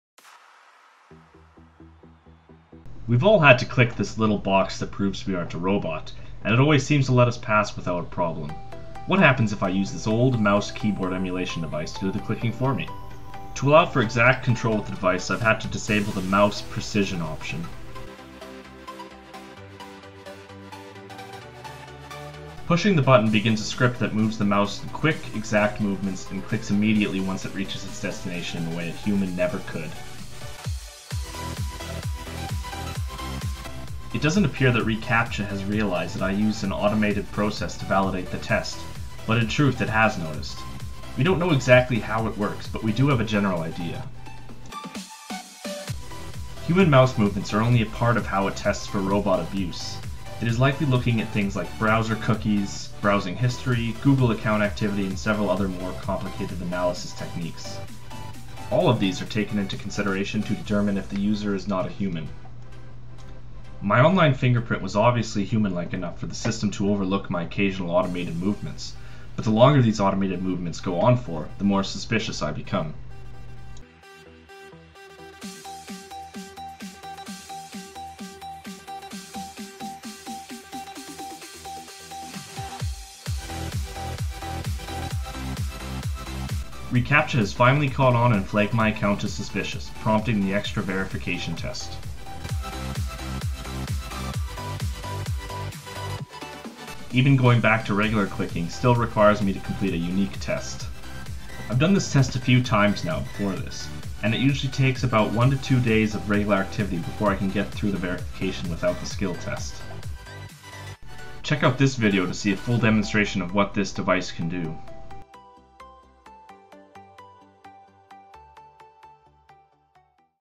I'm not a robot: Arduino Micro vs reCAPTCHA
Ever wonder what it takes to make google think you are a robot?